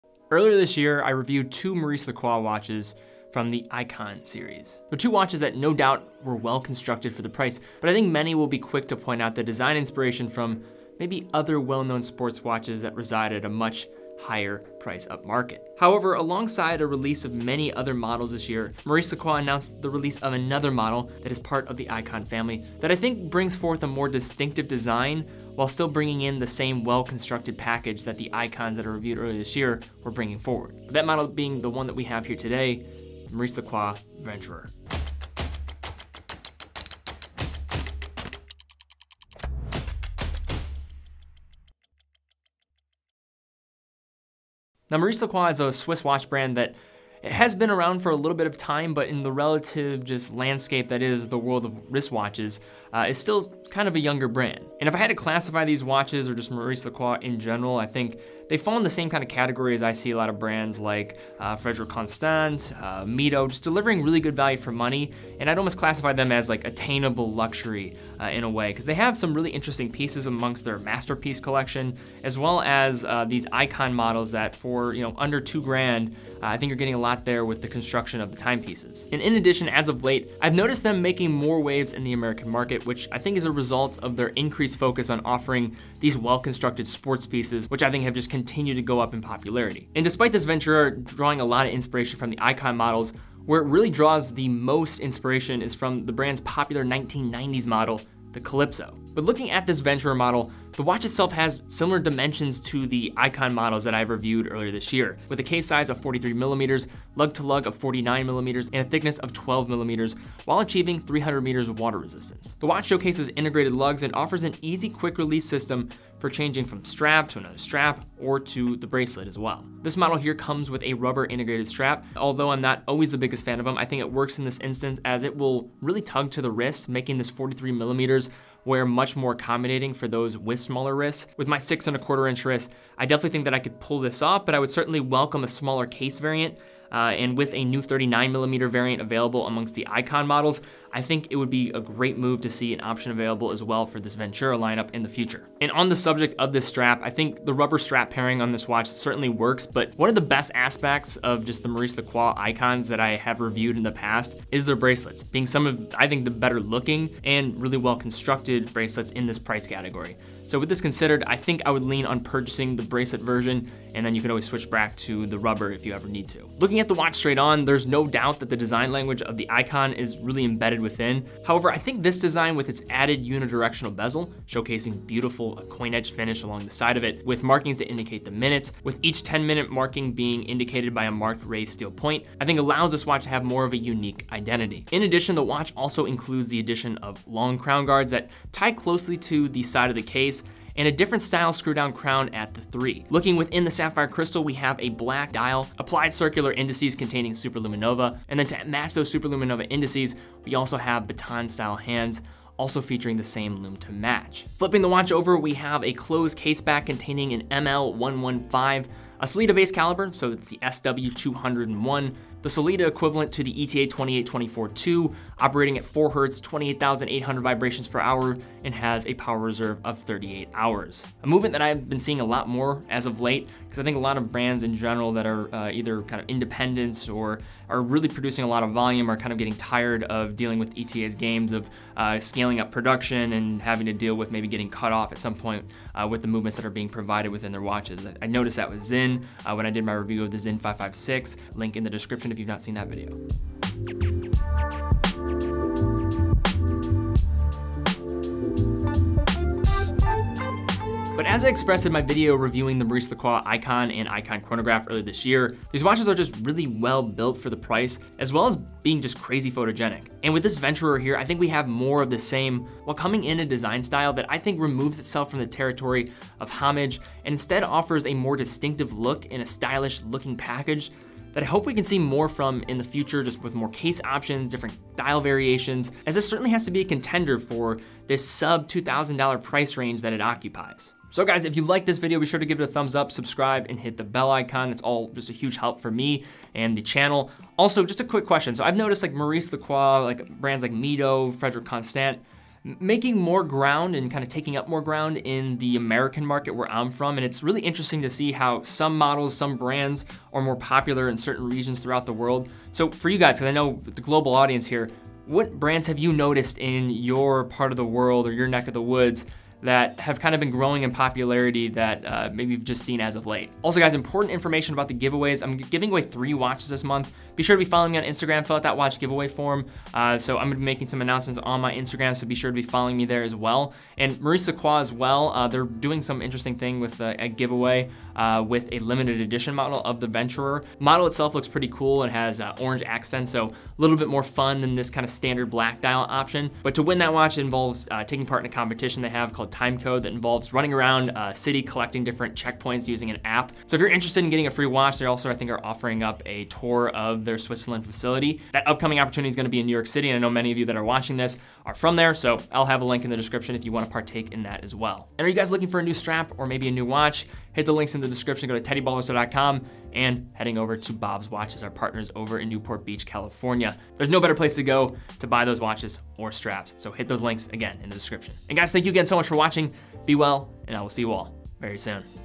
Maurice Lacroix Venturer - A Stylish New Model In The Aikon Series (Hands-On Review)
Earlier this year, I reviewed two Maurice Lacroix Aikon models, the Aikon Automatic and Aikon Chronograph (Link below if you have yet to see it). Two watches that I enjoyed and was impressed with the finishing for the price, but I think many will be quick to point out the design inspiration from other well-known sports watches that reside much higher upmarket in terms of price.  However, Maurice Lacroix just announced a new model for this year that builds off of the Aikon line and their popular model from the 1990s, the Calypso.  In this video, we take a close look at the watch and I share my thoughts and hopes for the future from the model.